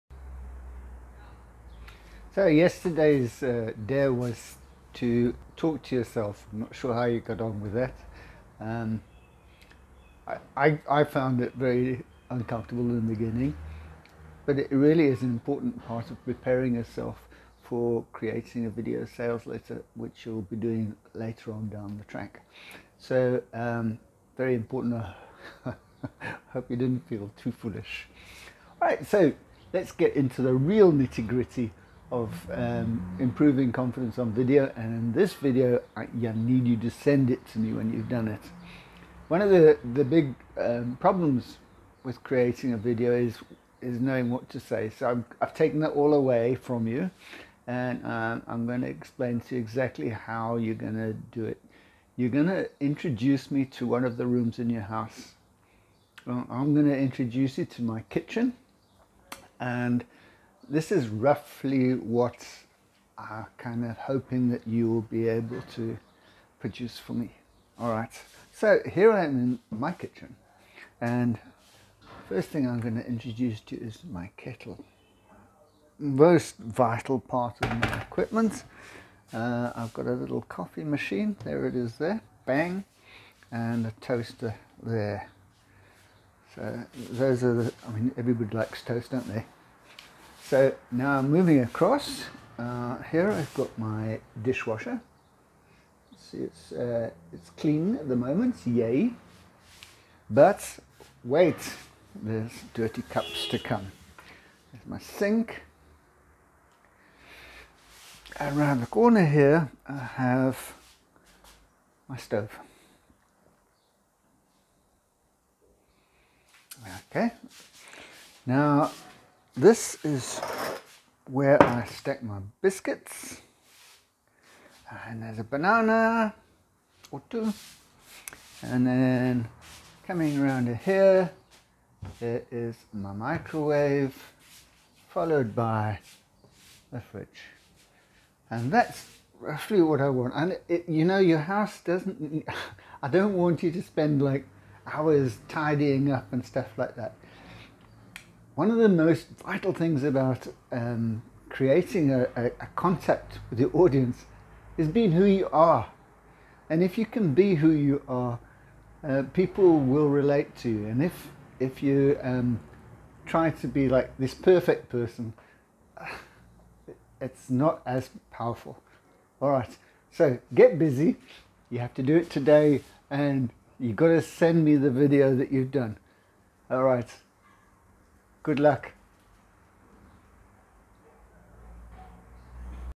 Kitchen Introduction.
In this video you are challenged to introduce me to a room in your house.  This is part of a confidence boosting plot designed to help you gain confidence in creating videos.